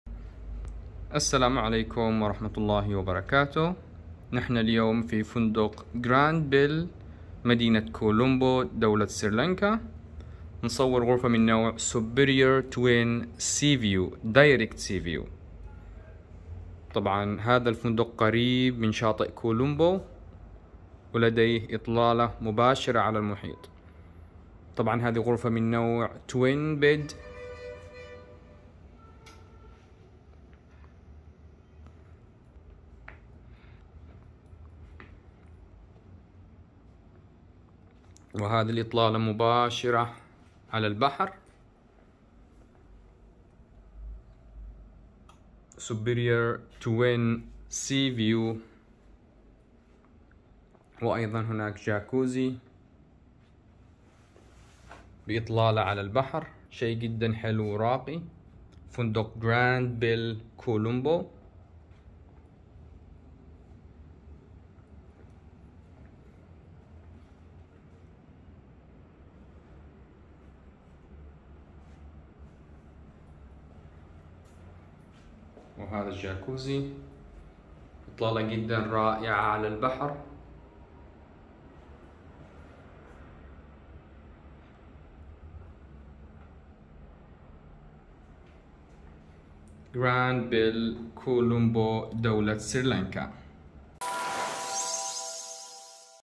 فنادق سريلانكا كولومبو | فندق جرانبيل كولومبو | غرفة سوبيرير سريررين منفصلين Granbell Hotel Colombo
افضل الفنادق في سريلانكا توجد في مدينة كولومبو حيث تتميز بمعماريتها الجميلة التي تنسجم مع الطبيعة المحيطة بها لتعطينا صورة فنية غاية في الروعة تلفت الأنظار وتأسر الألباب وتجذب السياح الذين يذهبون الى سريلانكا, وتشتهر فنادق كولمبو سريلانكا باطلالاتها الساحره على شواطئ المحيط والغابات الطبيعية, ولاننسى الخدمات الممتازة التي توفرها الفنادق لنزلائها والمطاعم التي توفر جميع انواع الماكولات لتناسب جميع الاذواق والاعمار.
ومن احد هذه الفنادق يعتبر فندق جرانبيل كولومبو من افضل الفنادق في كولومبو, يتميز بتصميمه الجميل وغرفه الواسعه ذات الاثاث الحديث والاطلالات الجميلة على المحيط والطبيعة.
غرفة سوبيرير نوع من انواع الغرف في فندق جرانبيل كولومبو يحتوي على سريرين منفردين, ويوجد مع الغرفة اطلالة كاملة على البحر رائعة جدا.

للحجز والاستفسار تواصلوا معنا عن طريق الوتس اب على الرقم ( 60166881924+ - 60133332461+ )  او تواصلوا معنا على الواتس اب عبر الرابط